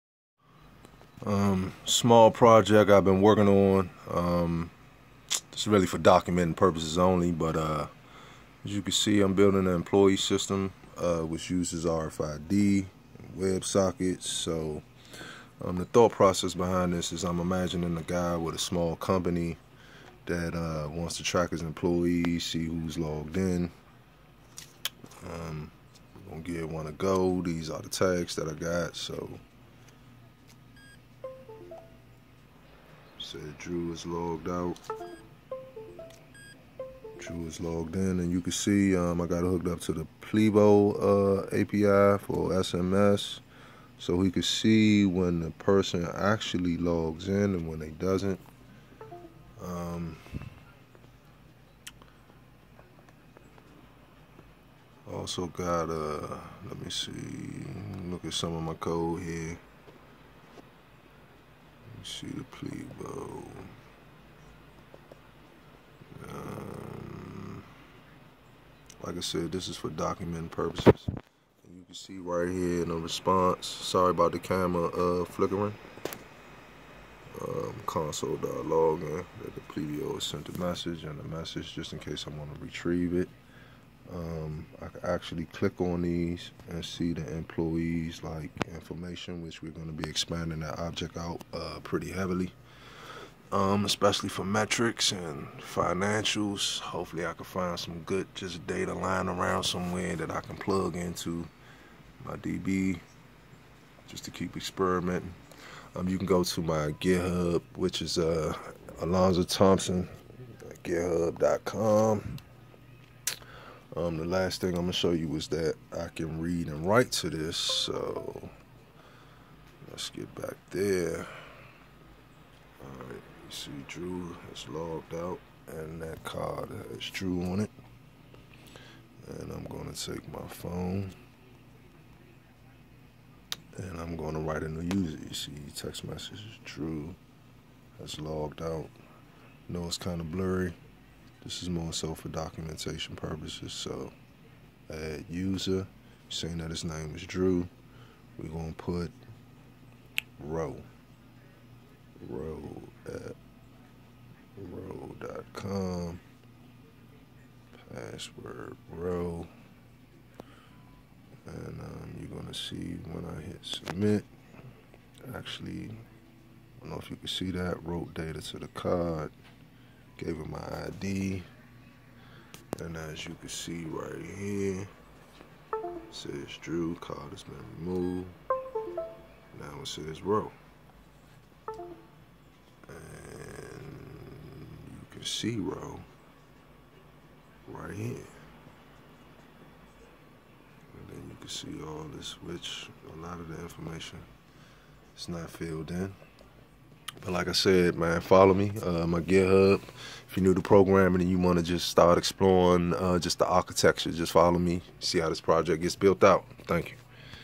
RFID Nodejs Websockets Plivo MongoDB
This is a video documenting my process on using rfid, Node js, Websockets, Smartcard js, MongoDb and Plivo. This is an ongoing project where we will be expanding out the employee object, with most of the fields that an employee would have in a database.  Also we will be creating company history for metrics.